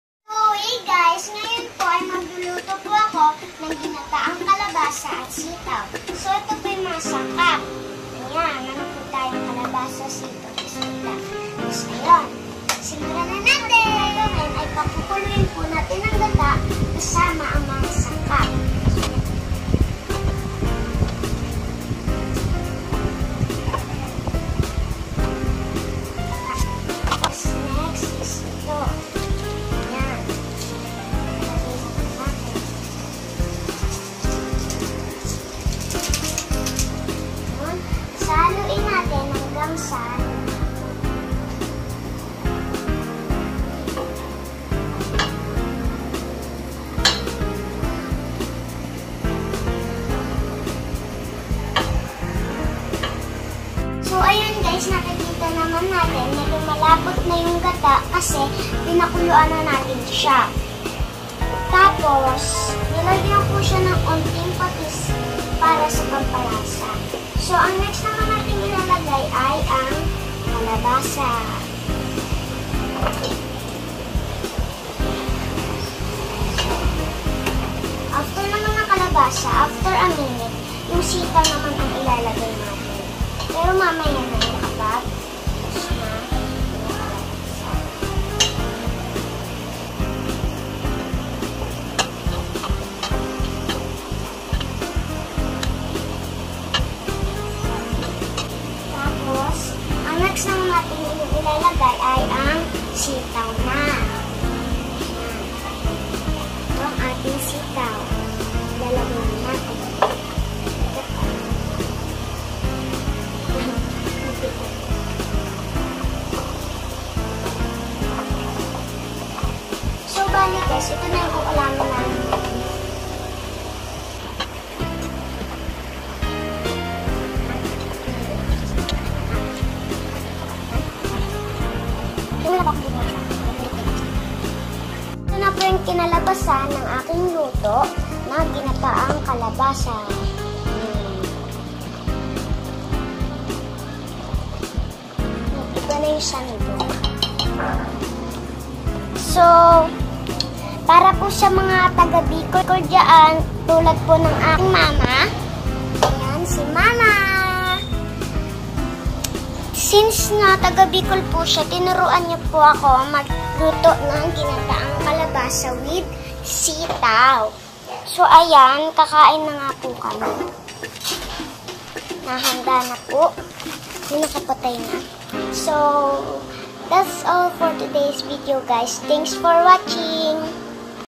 Ginataang kalabasa with sitaw ala Wejia
My 1st time cooking experienced😛😜😝